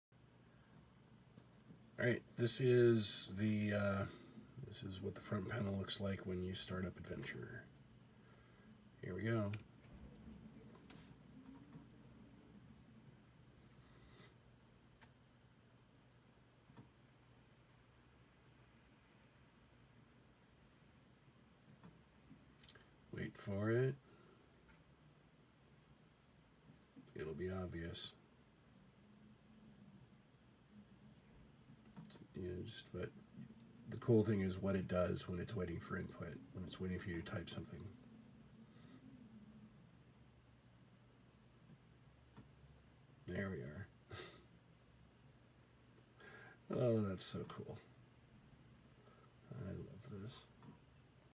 PiDP-8 startup of Adventure
This is the how the lights on the PiDP-8 respond when starting up the semi-original version of Adventure